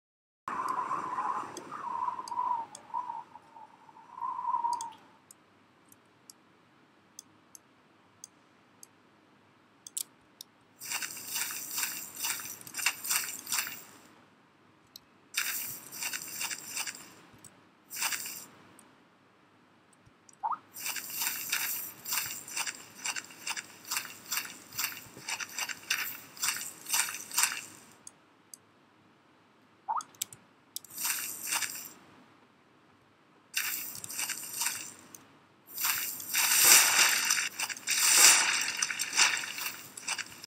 Defend your planet with magic walls
Starting from today, we're going to share with you the new content videos we're currently adding to the game.

So, here are magic walls. They are designed to protect your city from other players incoming attacks.
Two upgradable pillars create a field between them. You, as an owner can easily go through that field, but your enemies can't.